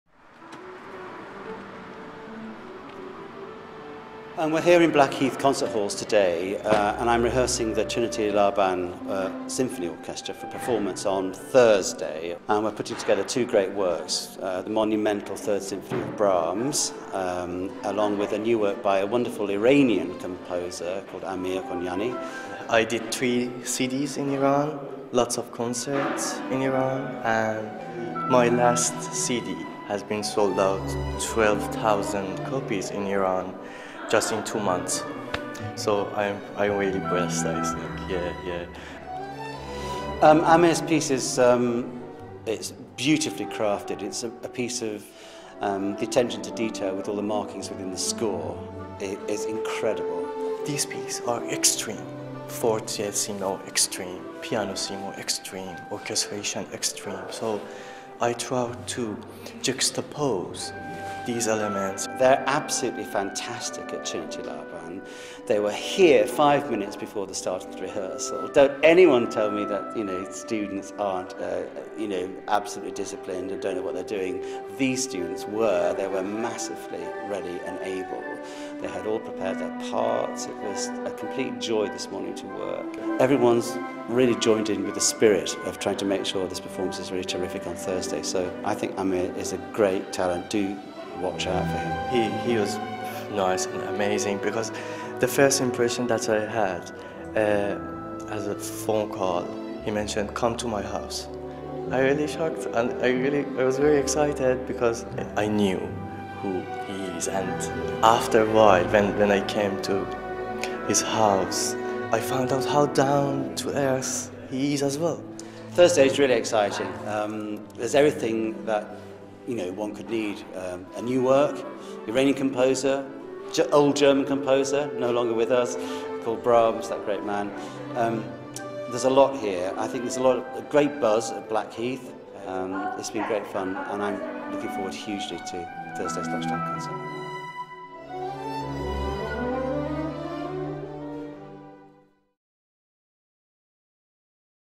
Peter Manning and Trinity Laban
Peter Manning conducts the Trinity Laban Concert orchestra at Blackheath Halls on November 4th 2010. Repertoire includes new work from up and coming Iranian composer Amir Konjani.